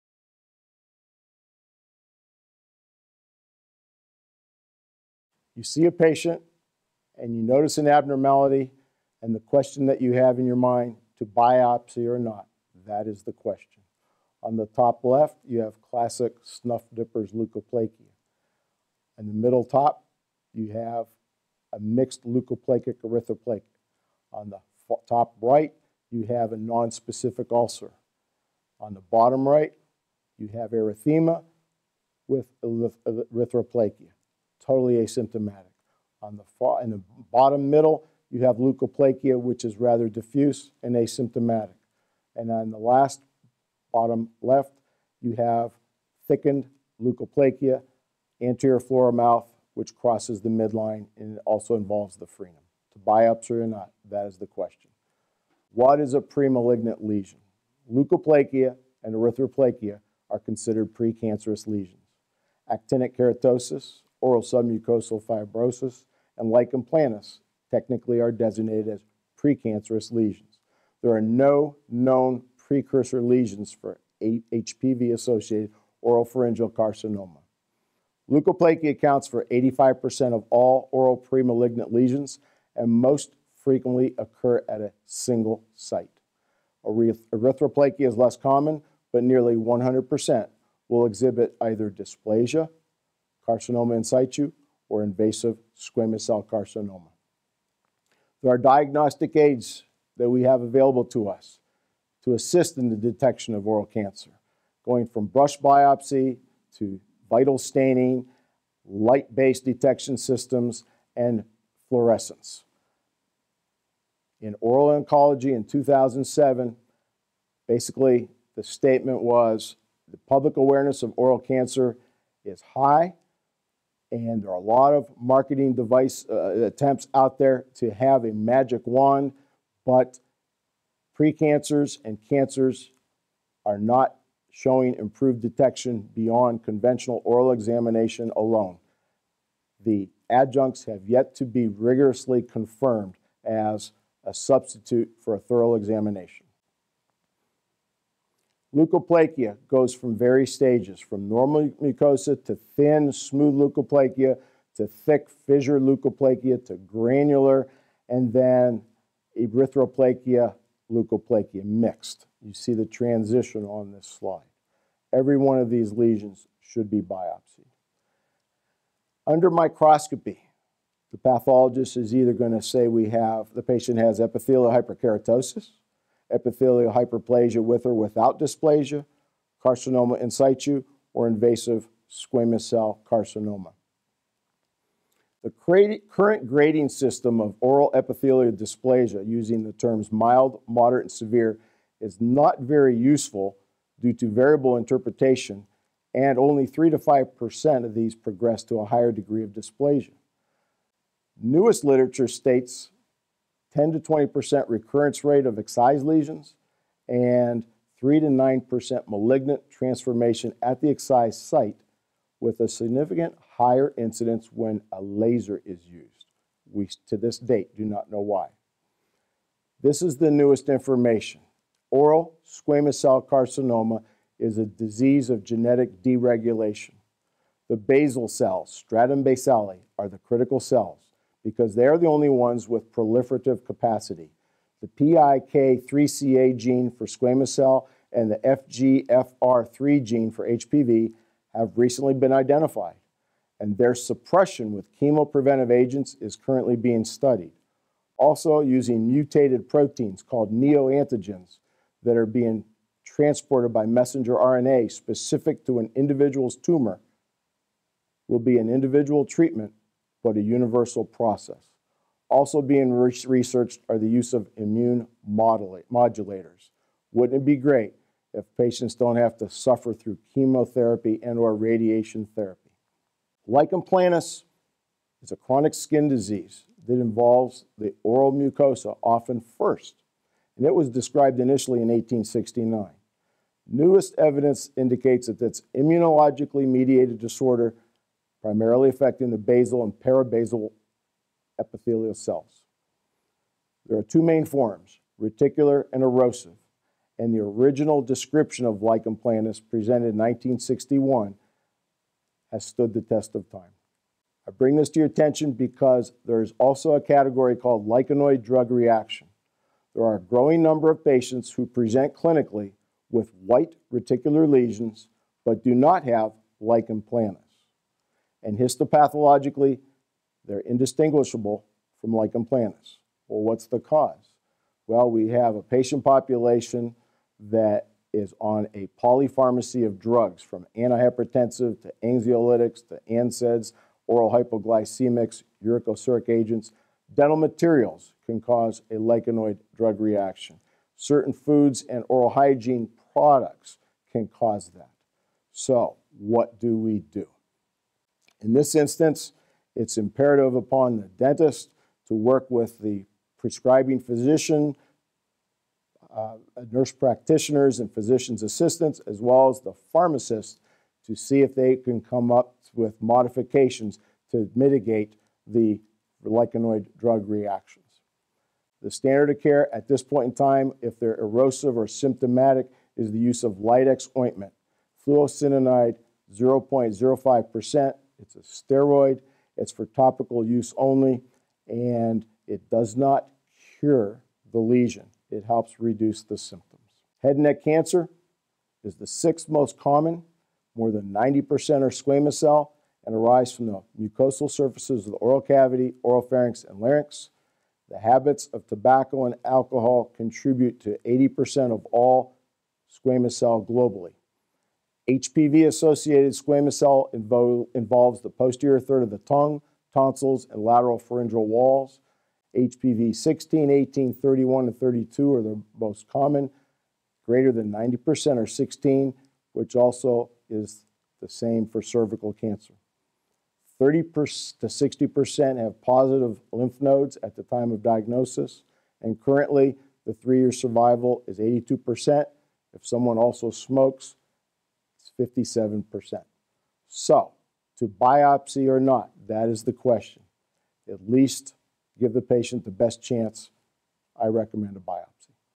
To Biopsy or Not? Examining Oral Lesions
John Alonge, MS, DDS reviews situations for which conducting a biopsy for potentially malignant lesions is recommended.
For ADA CE online, and Dr. Alonge's course on Minimally Invasive Exodontia Techniques